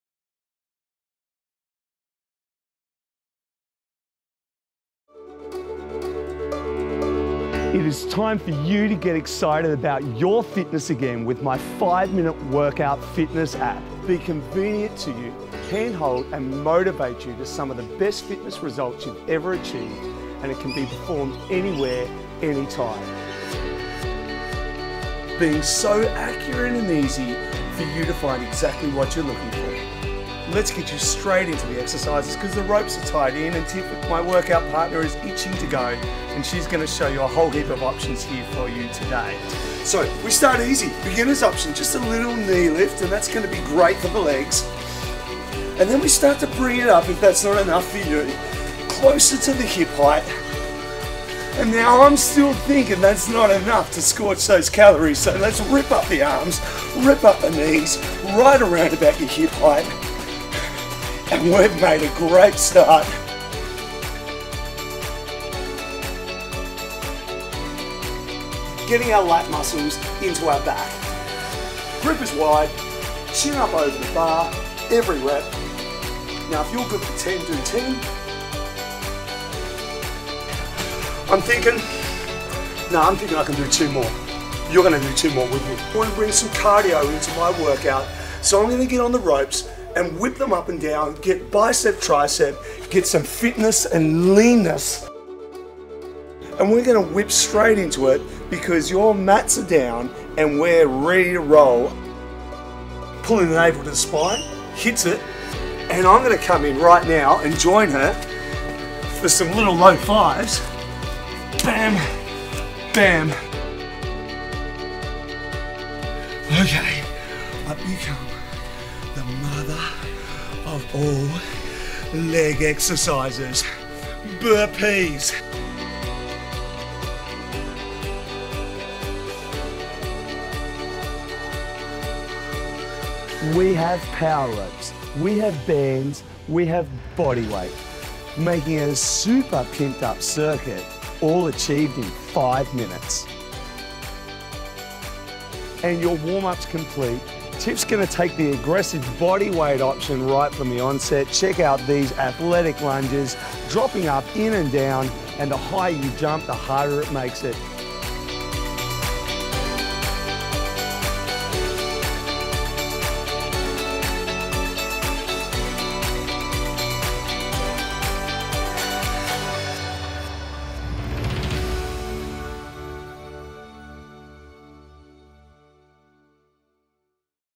Luke Heath Fitness Presenter Showreel
Luke Heath is a highly regarded Australian Fitness Professional and Entrepreneur, Creator of his own Group Fitness programs, National Group Fitness Trainer and Presenter, Presenter of Luke Heath 5 Minute Workout Series, Personal Trainer and athlete.
Luke 'walks the talk', he's raw and very real in every way and leads the way in every aspect of his fitness career. His passion, motivation, drive and inspiration shines throughout every aspect of his life. Mentoring, challenging, motivating and inspiring others every day to incorporate enjoyable and efficient fitness activities into their lifestyle. It's his overwhelmingly passionate personality and admirable presentation abilities that draw crowds to his fitness sessions and group fitness programs.